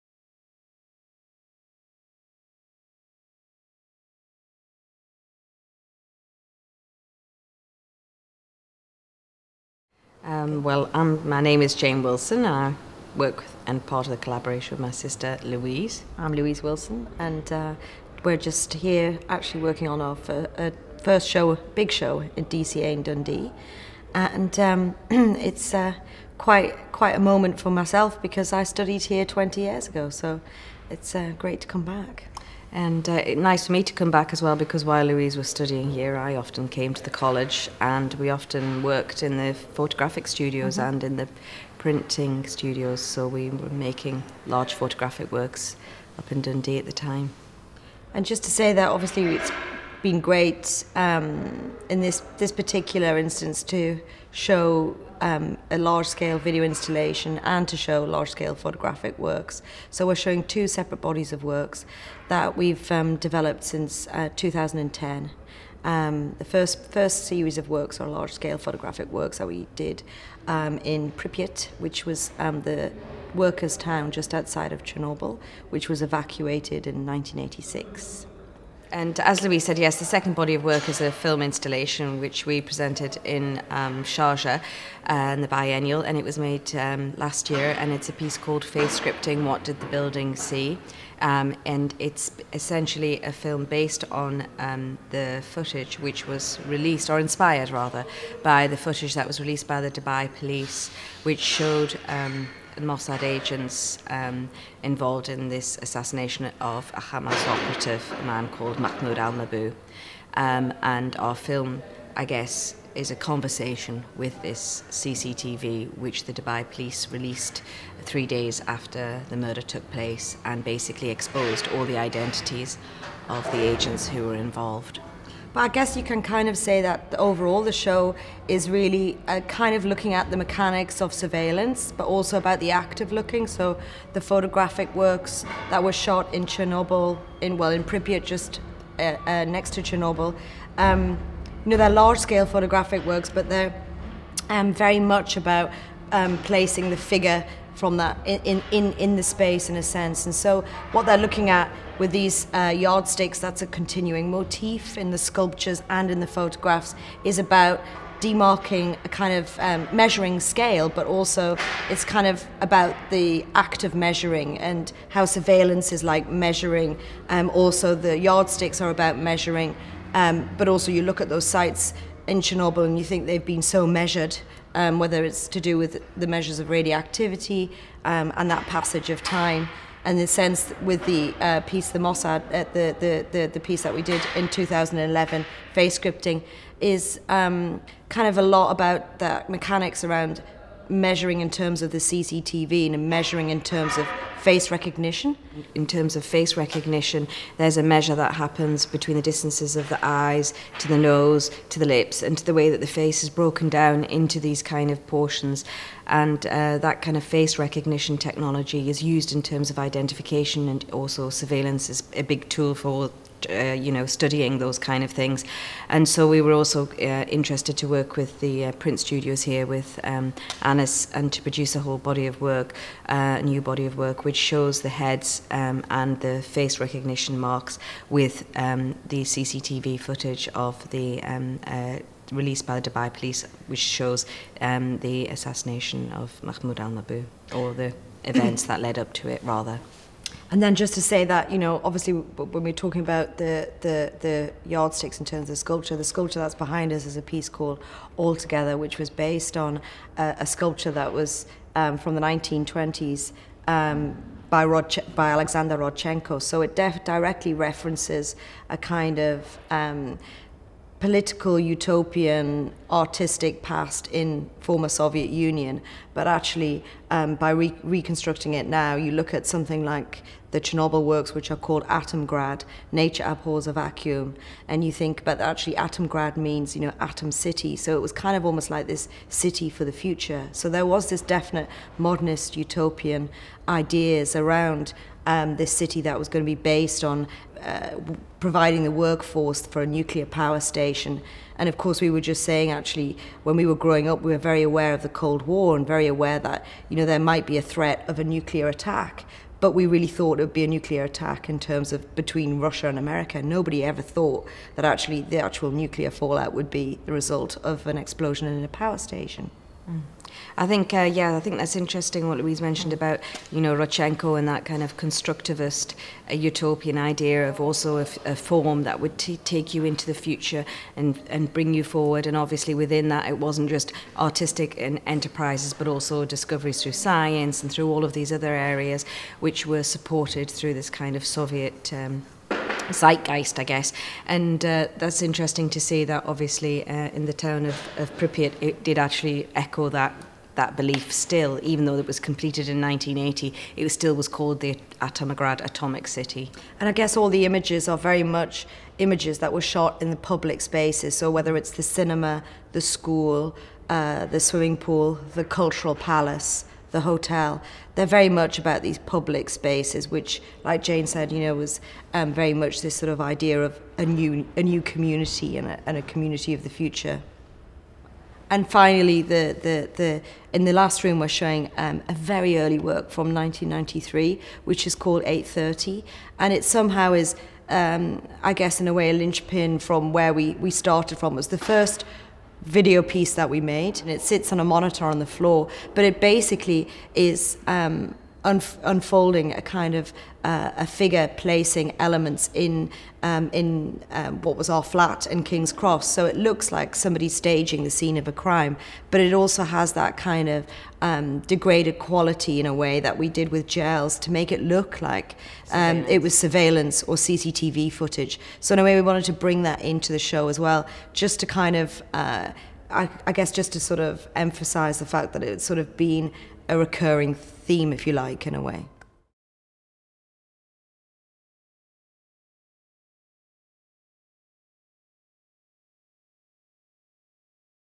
Jane and Louise Wilson
This exhibition brings together two bodies of recent work by acclaimed British artists Jane and Louise Wilson: Atomgrad (Nature Abhors a Vacuum), 2010 and the UK premiere of Face Scripting -- What Did the Building See?, 2011.